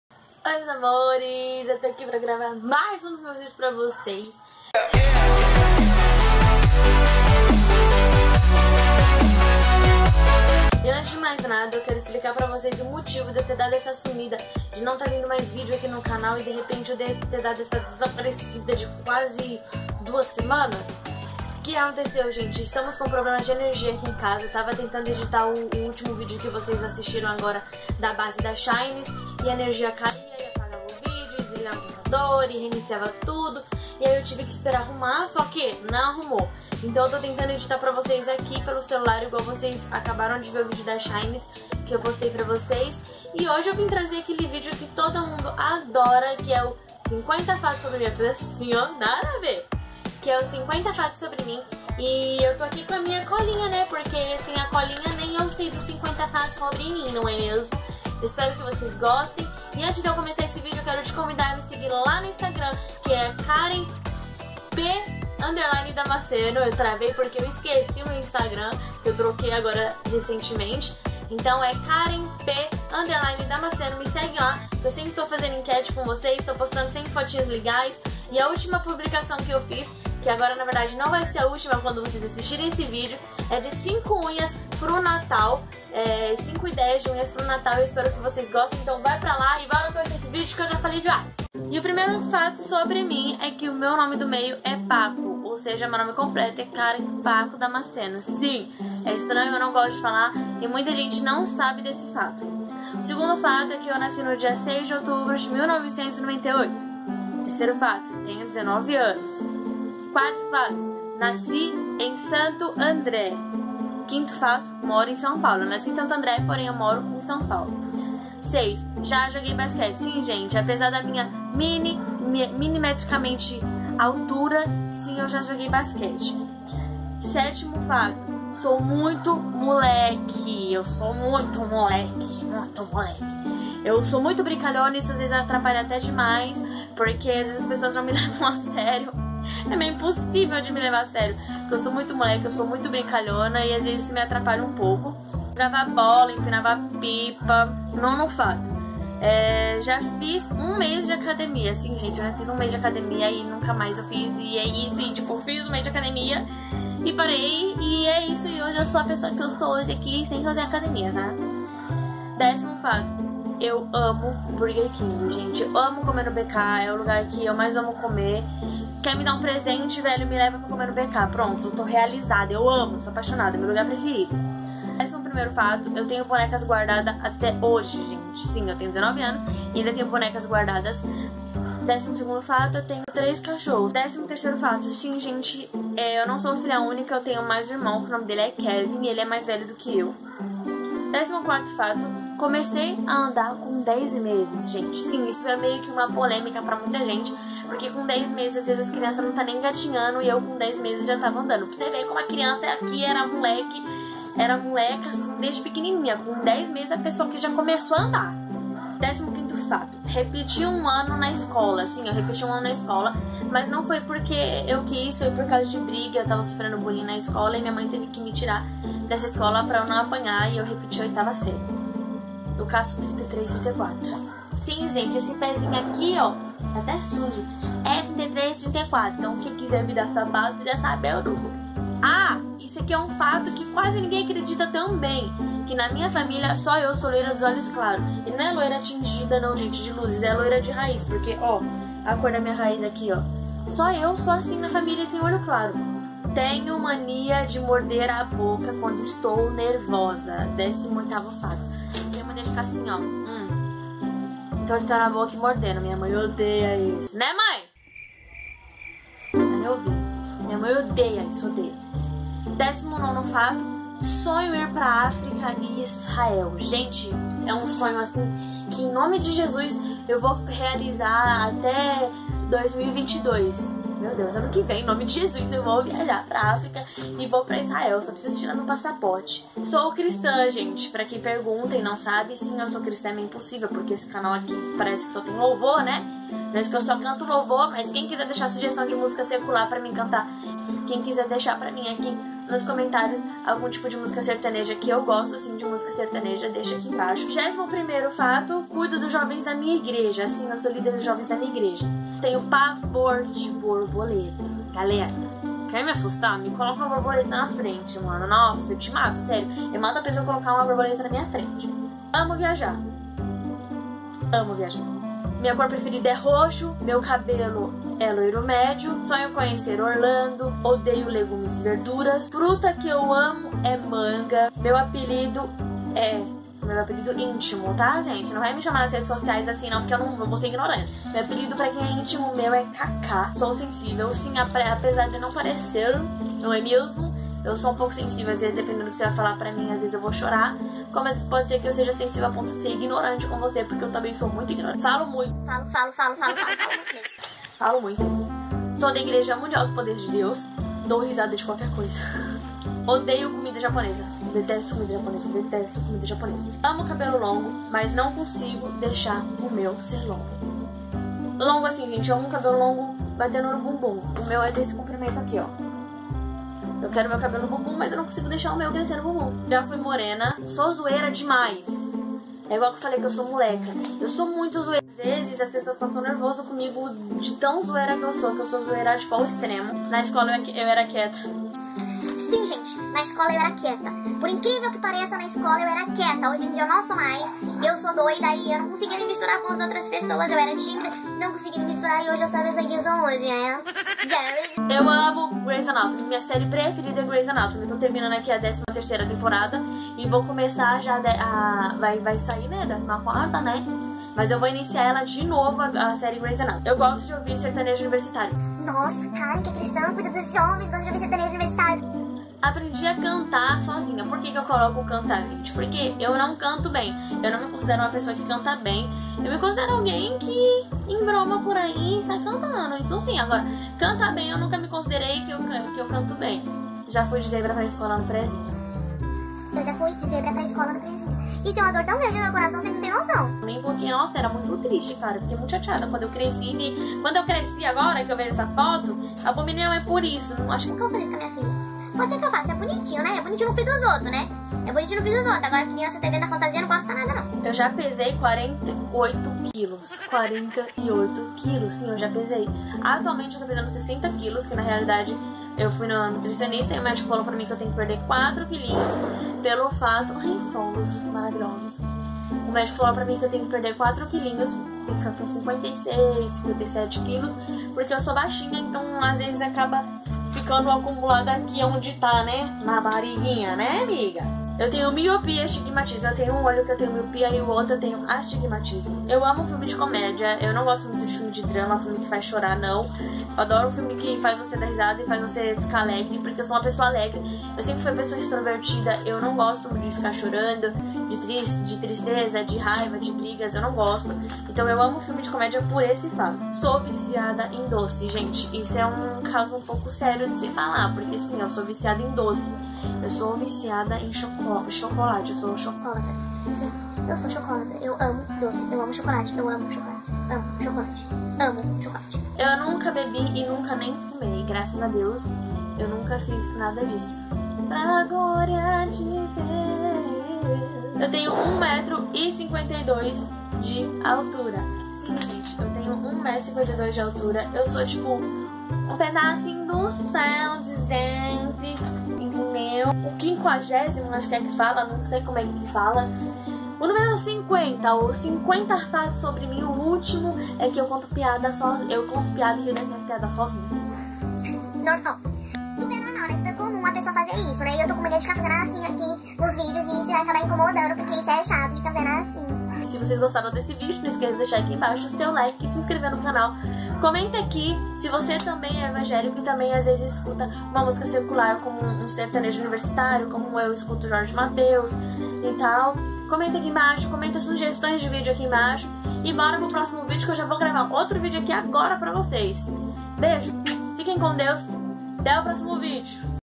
#50 FATOS SOBRE MIM| KAREN DAMACENO
fala Galera, vídeo novo para voces, agora são 50 fatos sobre mim.. Espero que gostem, deixem aqui aquele like maroto para ajudar na divulgação do nosso canal, que agora vai ter muito conteudo, para voces. ❤😍👌